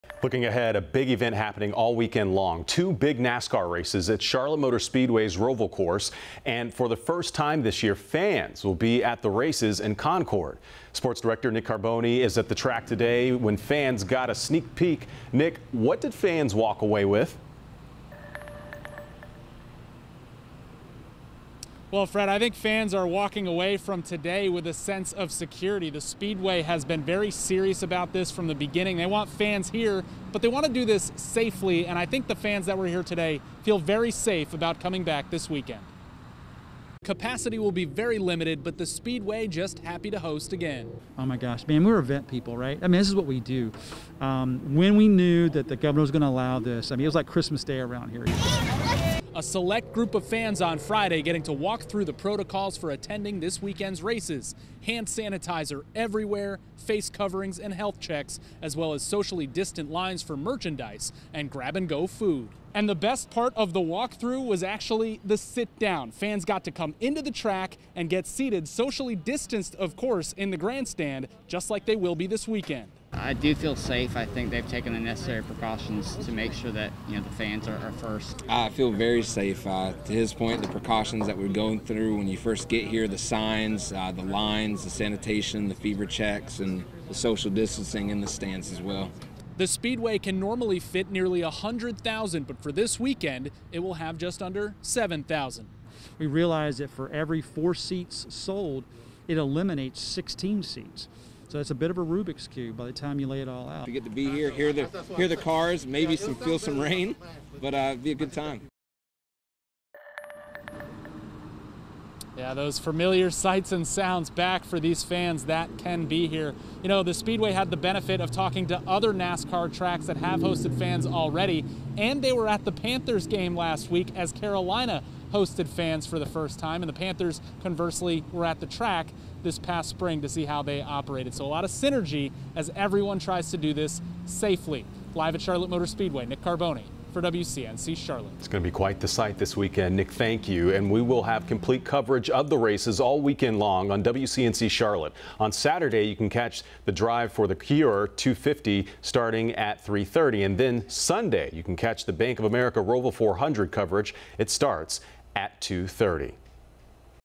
Fans return to Charlotte Motor Speedway
Before it hosts fans for the first time in 2020, Charlotte Motor Speedway invited a small select group of spectators to walk through some of the enhanced safety measures in place for this weekend's races.

On Friday, about two dozen fans saw how everything from intake, to concessions, to seating will work at NASCAR's ROVAL races on Saturday and Sunday.